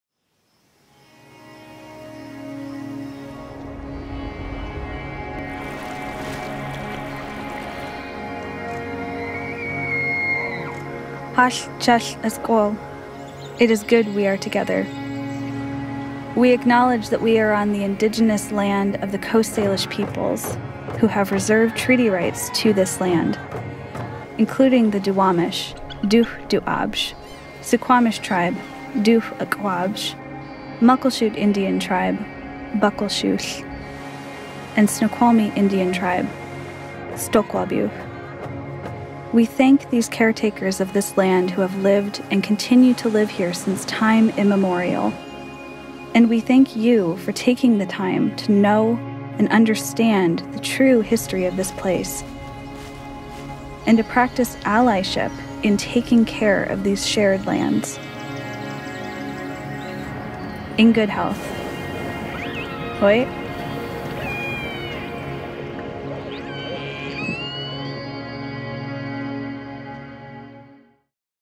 SIFF Land Acknowledgment
Created for the 2021 Seattle International Film Festival in partnership with  Nia Tero and the Snoqualmie Indian Tribe, to precede all virtual and COVID-safe in-person screenings in the Seattle region, this short film narrated by McKenna Sweet Dorman (Snoqualmie) shows the beauty of the Pacific Northwest and the many beings who exist here, and pays homage to the Coast Salish peoples who have called this place home since time immemorial. This includes the Duwamish (dxʷdəwʔabš), Suquamish Tribe, (dxʷəq̓ʷabš), Muckleshoot Indian Tribe (bəqəlšuł) and Snoqualmie Indian Tribe (sdukʷalbixʷ).